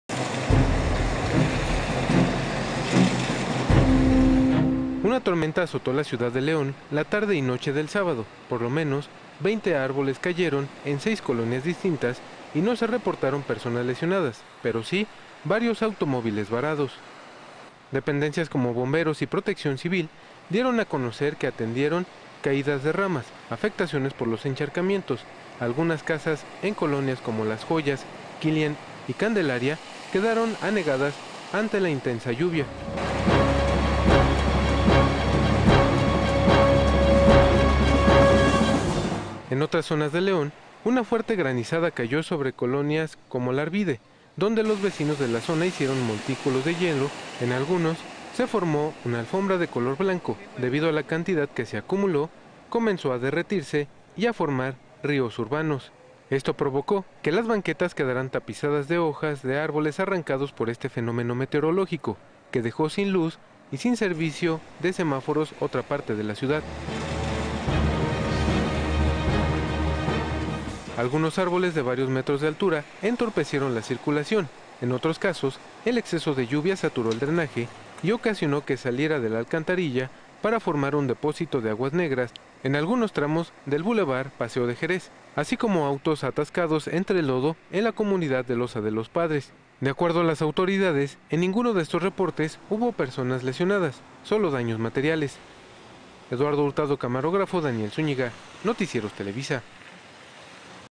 Bajio Noticias - Causó estragos la tormenta
La lluvia que cayó el fin de semana en León desquició la ciudad.